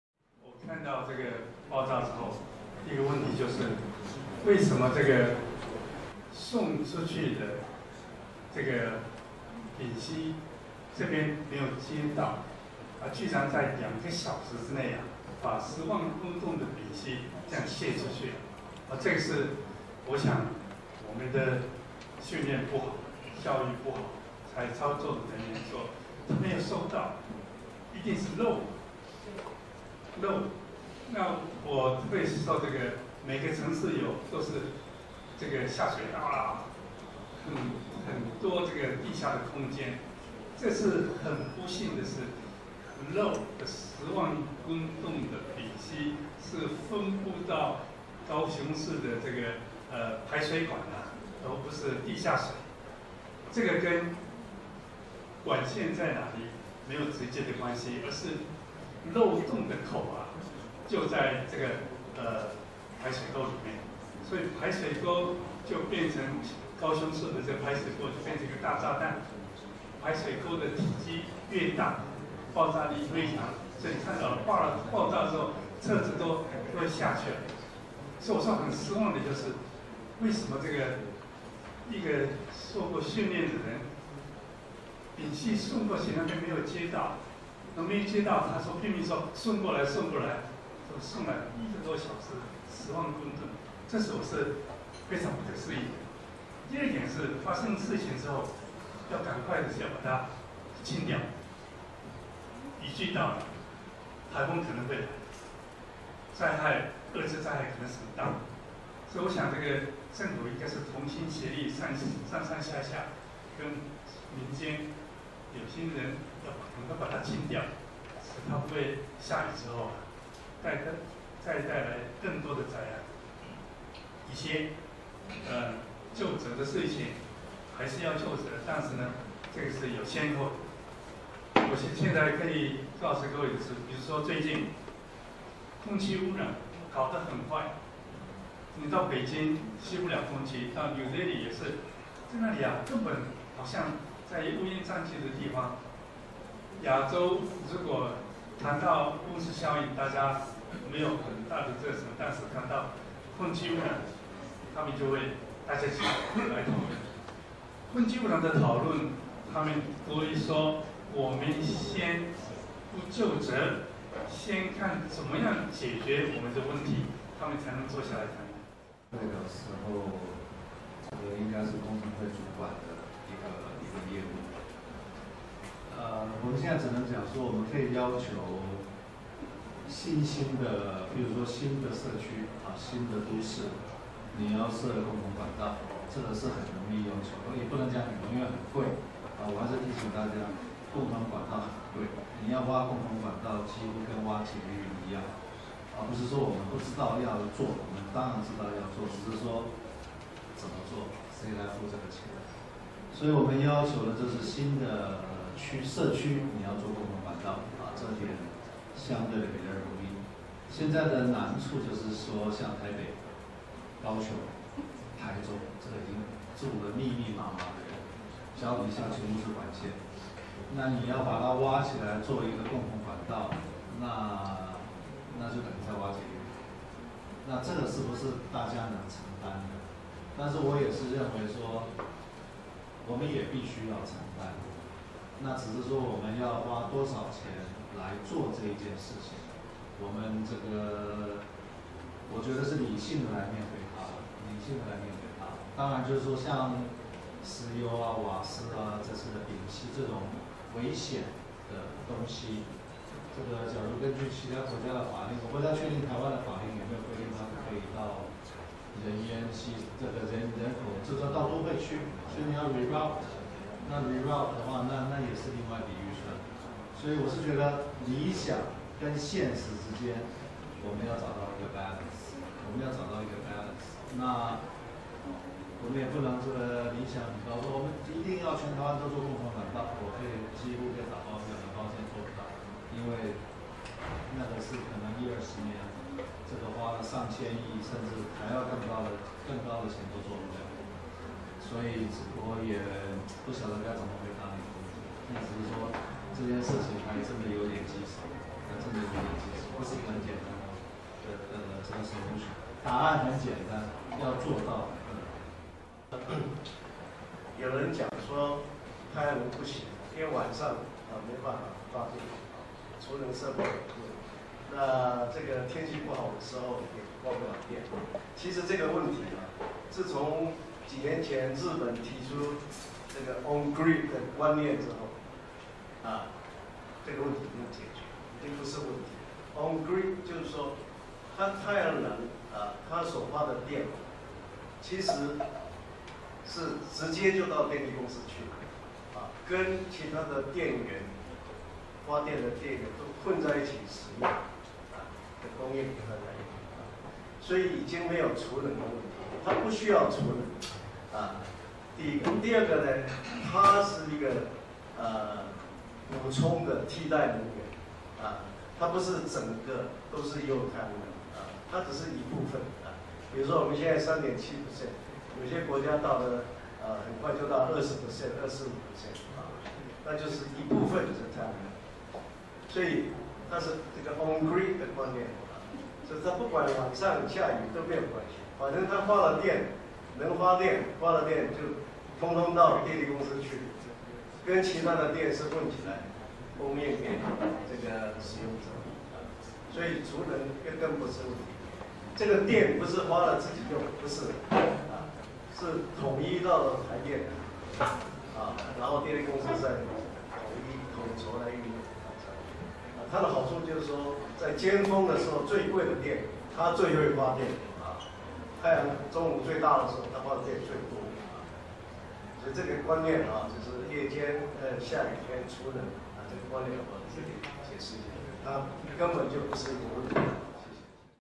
鉅變新視界沙龍：台灣永續發展轉型－共同管道與高雄氣爆事件檢討
由臺灣大學社會科學院風險社會與政策研究中心主辦，2014年8月9日（六）於華山文藝中心遠流別境舉行，沙龍主題：台灣永續發展轉型－國土、能源產業、社會轉型。

本段影片講者（按影片講話順序排序）：
李遠哲院士（前中央研究院長、中央研究院院士）
李鴻源教授（內政部前部長、臺灣大學土木工程學系）
陳文咸教授（台灣太陽光電產業協會秘書長、南光科技顧問公司董事長）